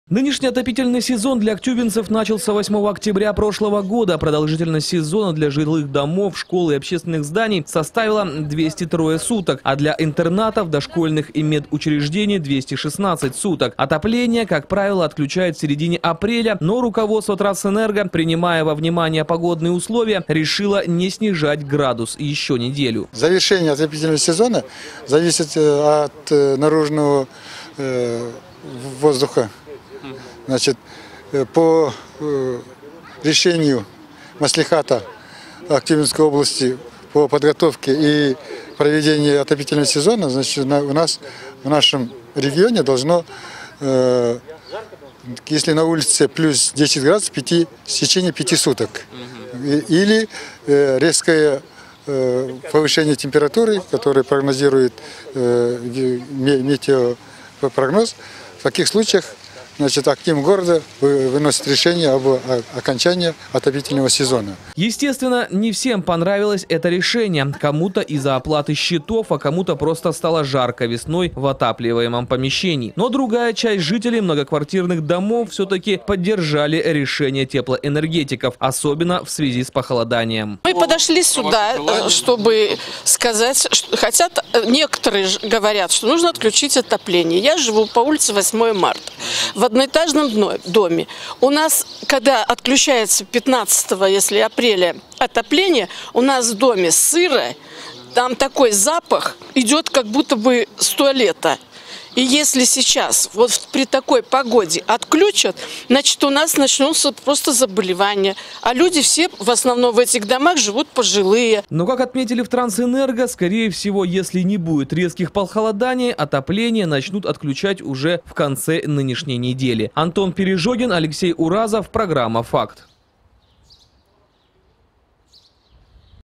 Отопительный сезон в этом году может продлиться до конца апреля
Отопительный сезон в этом году может продлиться до конца апреля. АО "Трансэнерго" связывает продление подачи отопления с прохладной погодой. Согласно правилам, если средняя температура воздуха составляет +10 и выше в течение 5 суток, руководство города может объявить о завершении отопительного сезона. Но на этой неделе прогнозируется снижение температуры по ночам до -5 по Цельсию. И такие метеоусловия, по информации теплоэнергетиков, продержаться как минимум до 22 апреля.
Нынешний отопительный сезон для актюбинцев начался 8 октября прошлого года. Продолжительность сезона для жилых домов, школ и общественных зданий составила 203 суток, а для интернатов, дошкольных и медучреждений - 216 суток. Отопление, как правило, отключают в середине апреля, но руководство трансэнерго, принимая во внимание погодные условия, решило не снижать градус еще неделю.
Естественно не всем понравилось это решение. Кому то из-за оплаты счетов, а кому то просто стало жарко весной в отапливаемом помещении. Но другая часть жителей многоквартирных домов все-таки поддержали решение теплоэнергетиков. Особенно с похолоданием.
Но как отметили в "Трансэнерго" скорее всего, если не будет резких похолоданий, отопление начнут отключать уже в конце нынешней недели.